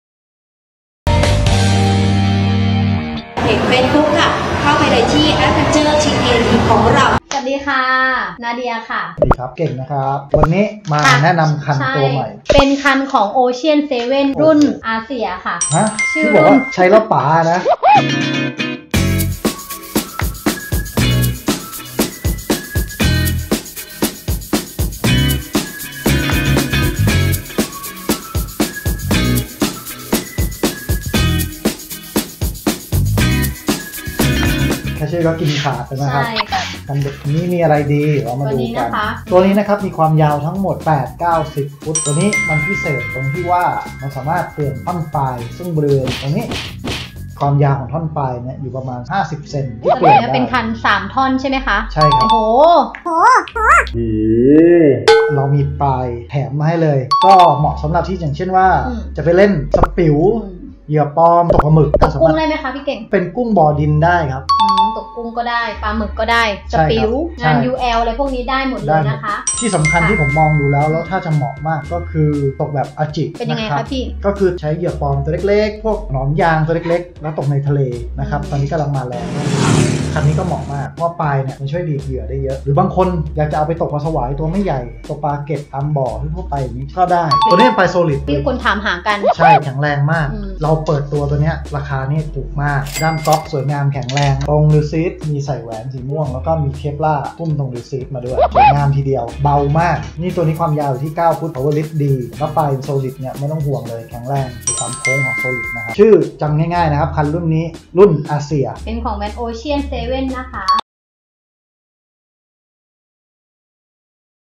โอเชี่ยน 7 อาเสี่ย แถมปลายโซริท ฟรี ให้อีก3 เวท คัน ปลายโซริท ถอดได้
นวัตกรรมใหม่ล่าสุดพลิกโฉมหน้า วงการตกปลา แค่เปลี่ยนปลาย// โอเชี่ยน7 อาเสี่ย แถมปลายโซริท ฟรี ให้อีก3 เวท คัน ปลายโซริท ถอดได้ 4900 ลดพิเศษเหลือเพียง 1900บาท แถมปลายสำรองให้อีกมูลค่า1000บาท มือสะปิ๋ว ยูแอล ตกหมึก มารวมตัวกันตรงนี้
คุณไม่ต้องเปลี่ยนน้ำหนักเหยื่อ เพียงแค่คุณเปลี่ยนปลายเท่านั้น การตกปลา จะเปลี่ยนไปทันที
ด้ามก็อกเต็มเบามาก เวทคันสุดครอบคลุม 8-20lbs
คัน2ท่อน คันมี 8ฟุต 9ฟุต 10ฟุต เท่านั้น ราคาเดียว 1900 บาท
ท่อนปลาย ที่ติดตั้งมาให้คือรหัส 50g แข็ง
ตัวปลายสุดที่แถมคือ
1. รหัส 50g 1อัน แข็ง เหมาะกับเหยื่อน้ำหนักเยอะสุด หรือใช้กับปลาปากแข็ง
2. รหัส 30g 1อัน กลาง
3. รหัส 15g 1อัน อ่อน

///เปิดเผยความลับเรื่อง ปลายโซริทโอเชี่ยนเซเว่น อาเสี่ย
แถมX CROSS เพิ่มความเหนียวคูณ2
OCEAN 7 2 ท่อน ปลายถอดได้ โดยปกติ ปลาโซริท จะอยู่ในเทคโนโลยี คันตัวท็อปราคาแพงจากค่ายญี่ปุ่น แต่นักออกแบบคนไทย ดูแลโปรเจคอาเสี่ยได้คิดค้น ปลายโซริท ได้เช่นกัน แถม ยังเป็น นวัตกรรมที่ให้ปลาย มาอีก3 อัน ด้วยคุณสมบัติ ที่
1 คงความเหนียวไว้เต็มที่
2 เพิ่มแอ็คชั่น ทำให้เหวี่ยงได้ไกล และแม่นยำขึ้น
3 ด้วยปลายที่ไว เมื่อปลากัดเหยื่อรับรองว่า รับแรงสั่นสะเทือนส่งมาถีงปลายนิ้วอย่างรวดเร็ว
4 เมื่อวัดปลา ปลายโซริท จะมีแรงดีดที่สูงมาก ทำให้วัดปลา ได้เต็มวง นั่นคือ ตะขอถ้าคม ทะลุแน่ ที่สำคัญ มีปลายให้อีก3อัน
5 ติดตั้ง ลายX CROSS เพิ่มความเหนียว เข้าไปทั้งคัน จนถึงปลายคันไกค์ตัวถึงโซริท
6 เทคโนโลยีตัวใหม่ที่คนออกแบบชาวไทยใส่ไปเต็มที่กับคันนี้ เนื้อแบลงค์กราไฟท์
7 การวางไกค์ ทรงK ตัวใหม่เป็นทรงเอกลักษณ์ จากเรา มั่นใจได้ว่า เหวี่ยงเหยื่อได้ไกล. เพิ่มแรงงัดปลาได้เต็มที่
8 การวางขนาดไกค์ที่พิถีพิถัน ทำให้เมื่อประกอบรอกและมีเหยื่อห้อยอยู่ รับรองว่า ปลายไม่ทิ่ม เหมือนคันที่มีปลายโซริท อื่นๆ
9 ไกค์ ซิลิคอน. รีลซีทชุดแต่งเต็มสูบ มีอะไหล่ท่อนปลายขายต่างหาก
10 น้ำหนักคันเบาหวิว ใส่รอกอะไรก็ดูหรูหรา สมราคา สวยงาม
ฝากติดตามชมคลิปมากมายได้ที่ youtube ช่อง Adventure TAD
⭐️ อย่าลืมกด subsribe และกดถูกใจด้วยนะจ๊ะ ⭐️
เพจ : Adventuretad อุปกรณ์ตกปลา
ไลน์ : @adventuretad